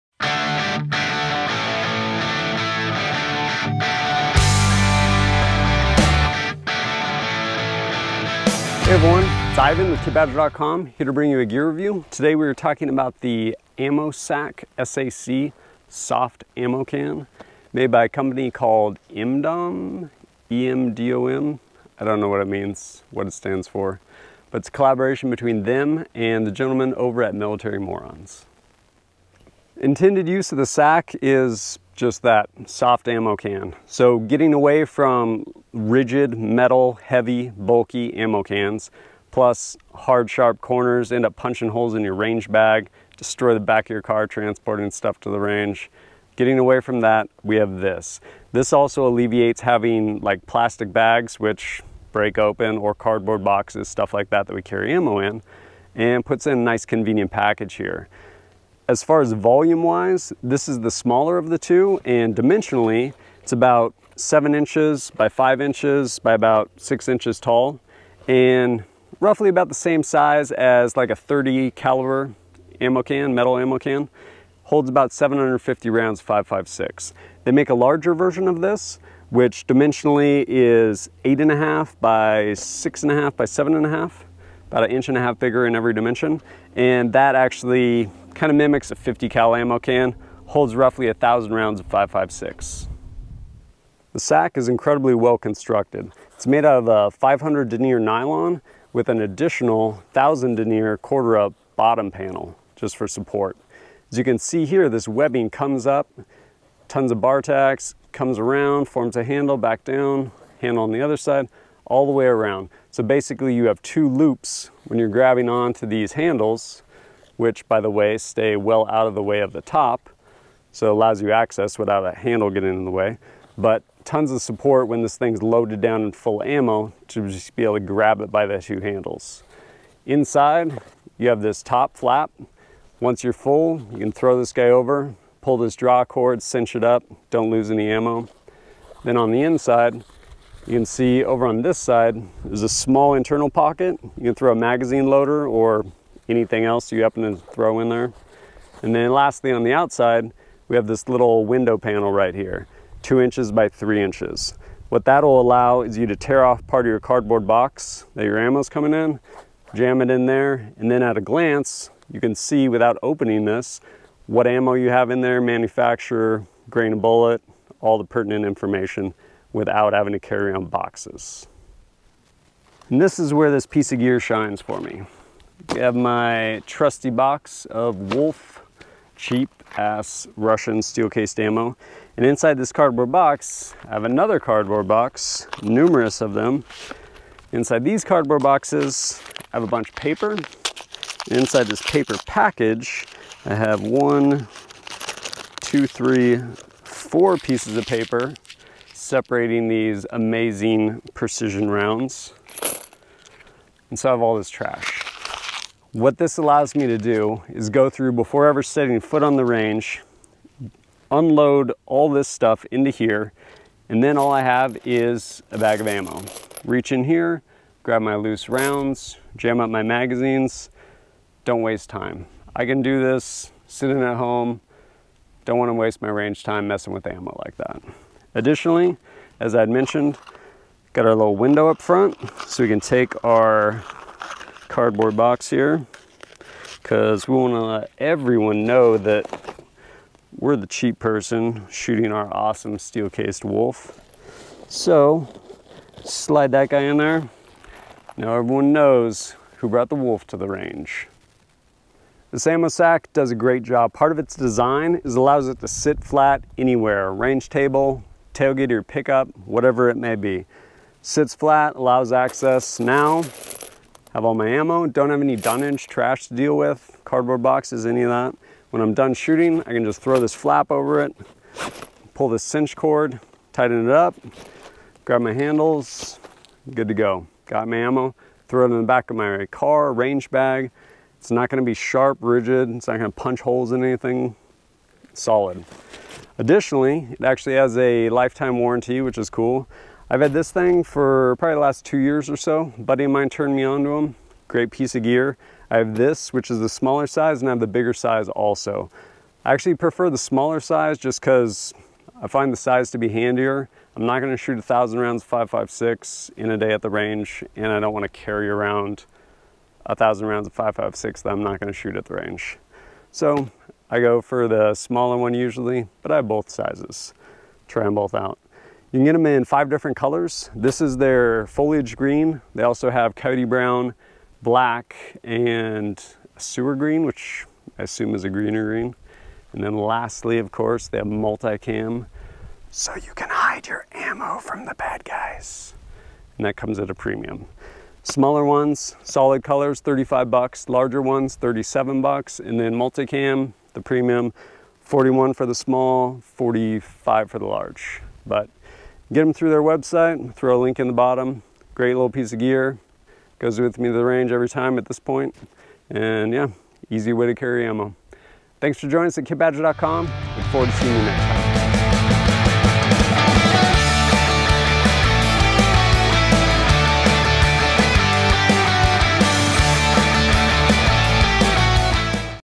Ammo S.A.C.
Lugging around metal ammo cans is a pain.  But so is having plastic bags and cardboard boxes explode in your range bag or vehicle.  EMDOM has a elegant solution.  The Ammo SAC (Soft Ammo Can).  It allows you to carry all of your ammo in a convenient and durable way.

Rifle, pistol and shotgun ammo, or just fill it with snacks...  The small size holds approximately 750 rounds of 5.56mm while the larger size holds about 1000 rounds of 5.56mm ammo.